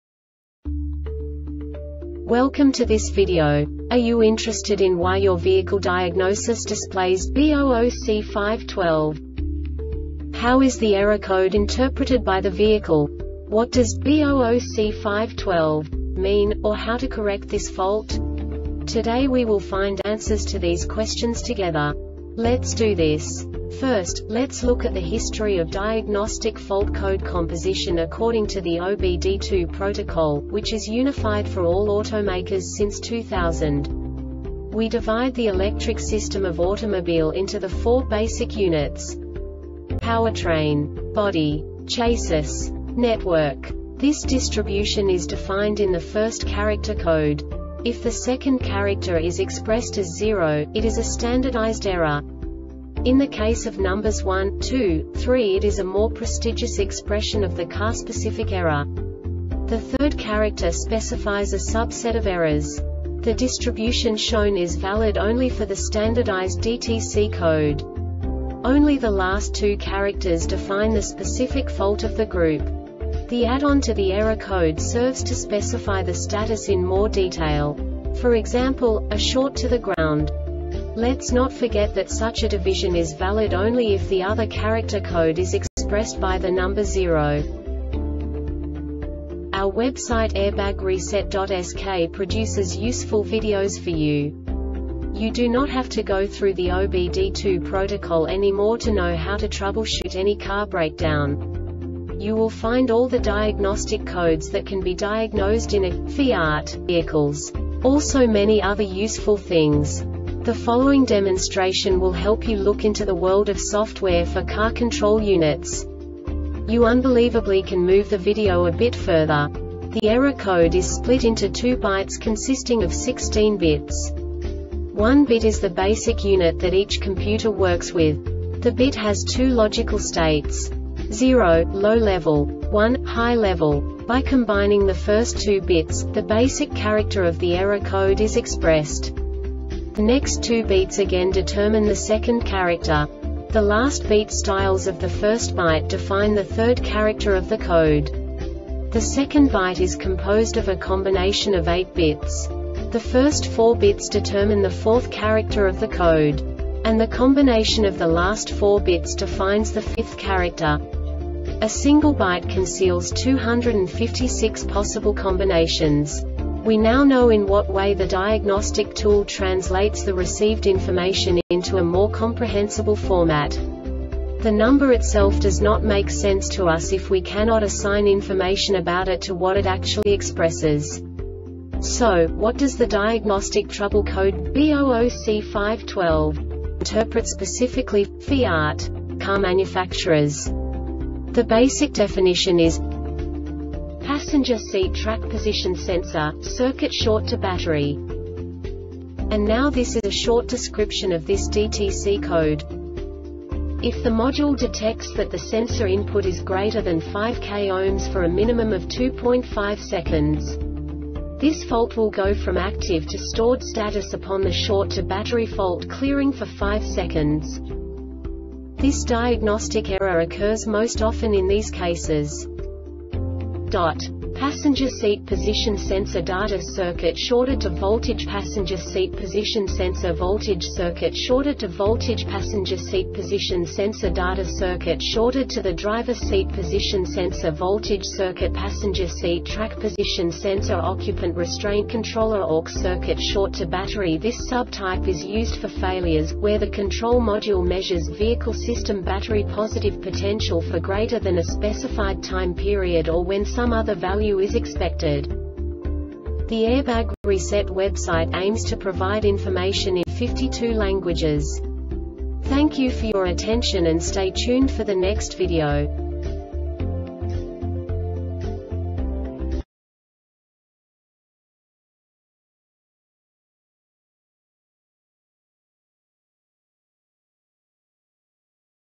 DTC Fiat B00C5-12 Short Explanation
Contents:
0:21 Basic DTC analysis according to OBD2 protocol standard.
1:48 Insight into programming
2:58 A diagnostically understandable description of the Diagnostic Trouble Code
3:05 Basic error definition

Make:
FIAT
Code:
B00C5-12
Definition:
Passenger Seat Track Position Sensor - Circuit Short To Battery
Description:
If the module detects that the sensor input is greater than 5K Ohms for a minimum of 2.5 seconds. This fault will go from active to stored status upon the short to battery fault clearing for 5 seconds.
Cause:
Passenger Seat Position Sensor Data Circuit Shorted To Voltage
Passenger Seat Position Sensor Voltage Circuit Shorted To Voltage
Passenger Seat Position Sensor Data Circuit Shorted To The Driver Seat Position Sensor Voltage Circuit
Passenger Seat Track Position Sensor
Occupant Restraint Controller (Orc)
Failure Type: